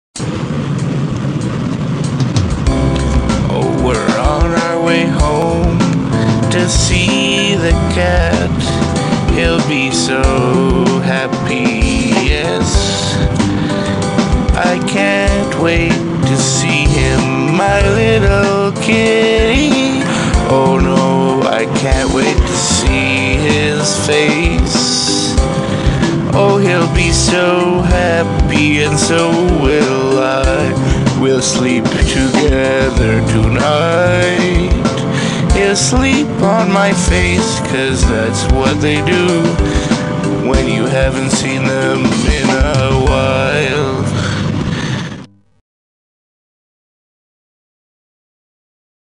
Kitty song [On our way home]
Made this on the way home from vacation after not seeing kitty for two weeks!